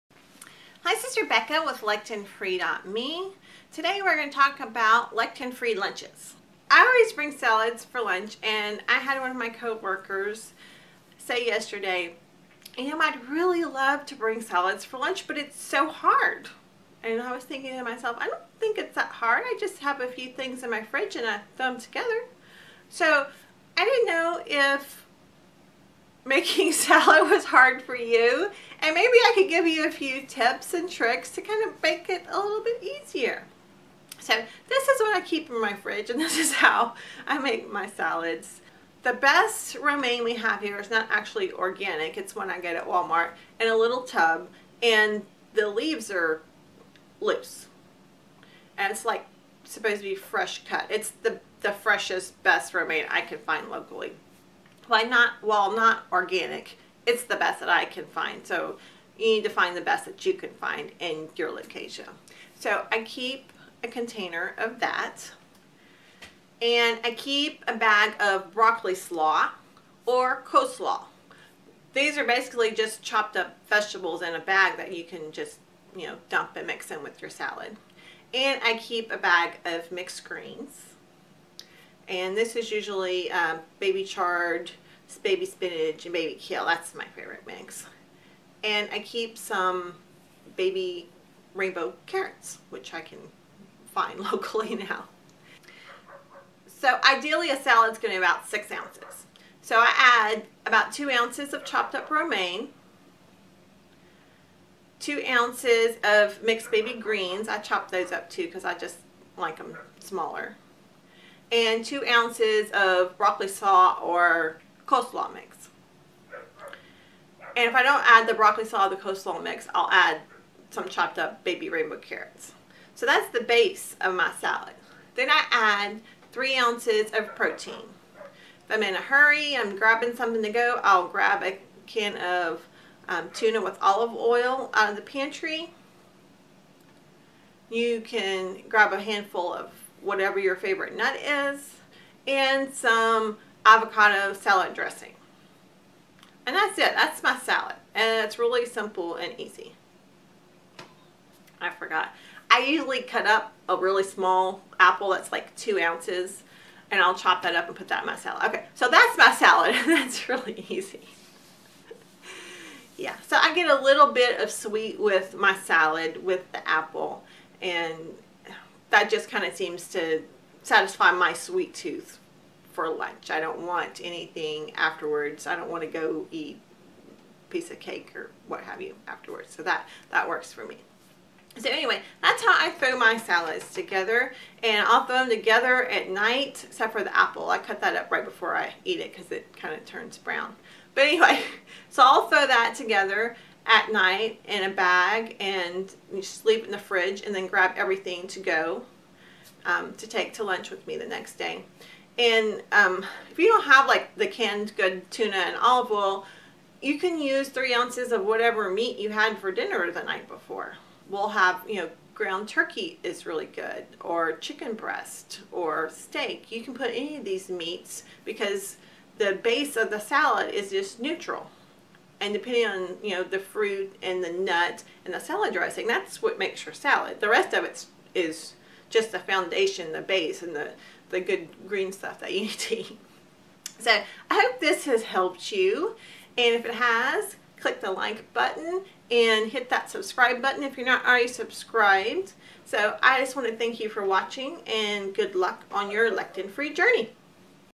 Lectin Free Foods| What to Eat for Lunch
Eating Lectin Free I usually have a salad for lunch.  A coworker remarked that she wanted to bring a salad for lunch but it was too hard.

So I thought I would share my easy salad formula so you can throw a salad together for your lunch.

Ideally, a salad is 6 oz of greens.

I use:
2 oz of Romaine Lettuce
2 oz of mixed baby green
2 oz of broccoli slaw, coleslaw or baby carrots

I top that with 3 oz of tuna in olive oil
a handful of nuts
guacamole and avocado salad dressing
and a small 2 oz apple for a bit of sweet.

I hope this helps you to make a salad you can take for your lunch.

Good Luck on your Lectin Free Journey!

Rebecca